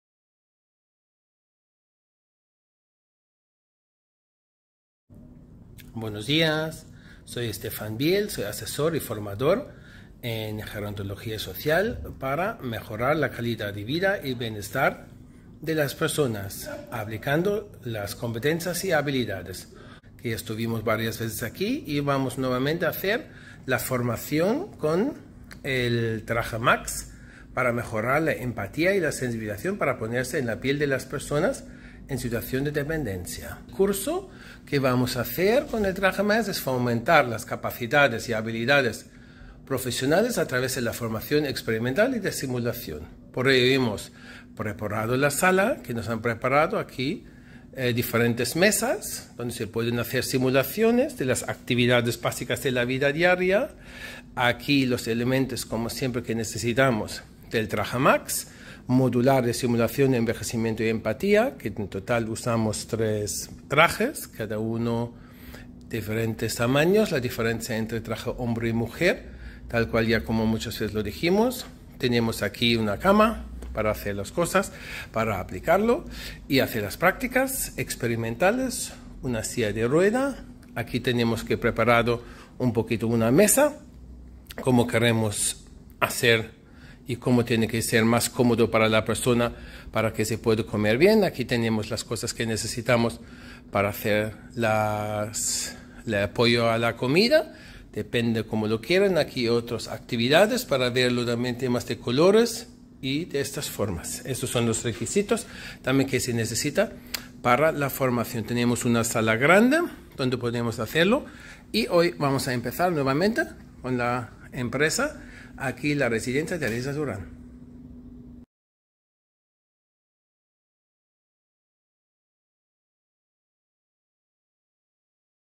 Mejorar la calidad de vida y el bienestar de las personas con el TrajeMax
Mejorar la calidad de vida y el bienestar de las personas en situación de dependencia, a través de la formación experimental y simulación, es el objetivo del uso del TrajeMax en esta ocasión. Este año estoy muy orgullos de la aceptación de esta formación tan enriquecedor, por ello el otro día tenía que comunicar que ya no quedan fechas para este año probar este taller.

Agradezco a la Fundació per a l'Atenció a Persones Dependents y la ResidenciaTeresaDuran y por supuesto a @CristinaMediano por contar de nuevo conmigo para poder aplicar la formaciónexperimental y de simulación con el TrajeMax. Tres años consecutivo apuestan por esta formación por su gran aceptación y satisfacción de la plantilla. En particular los profesionales de Auxiliares agradezcan probar esta experiencia única, y comunican en la reflexión el gran valor para su trabajo diario.